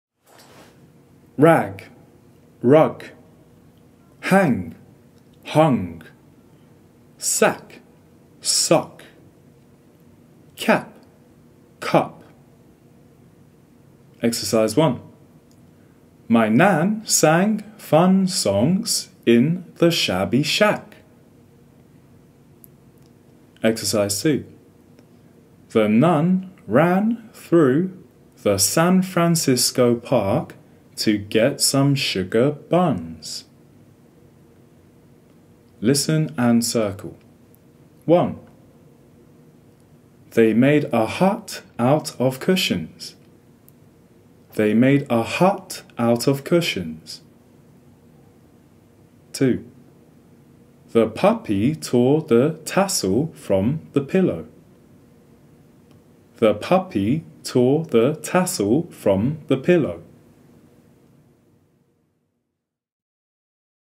Listening 初級 Wk 42 Pronunciation
This video is about A & U pronunciation